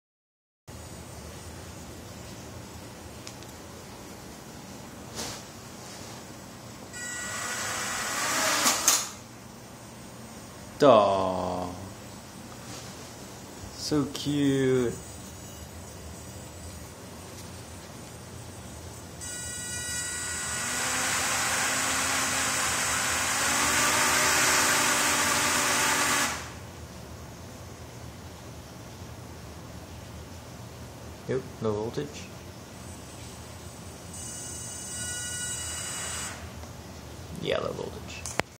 Kawaiicopter v1.3: First Mini Flight (Cute but Fail)
Kawaii copter takes its first 4 rotor hop but dies shortly after due to low battery. I am only using a 2s for this run.